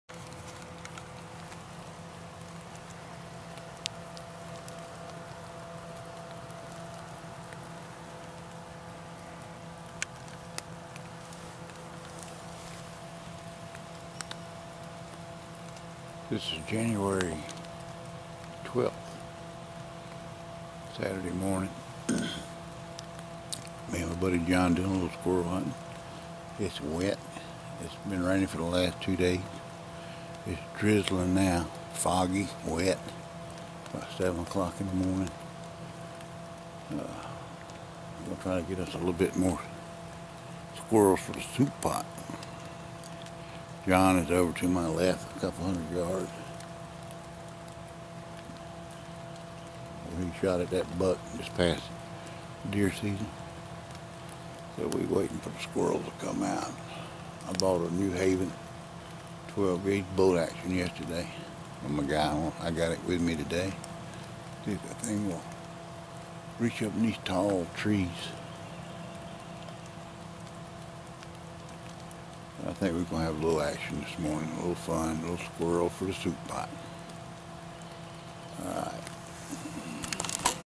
Early Morning Squirrel Hunt
Saturday Morning Squirrel hunting Jan 12th before daylight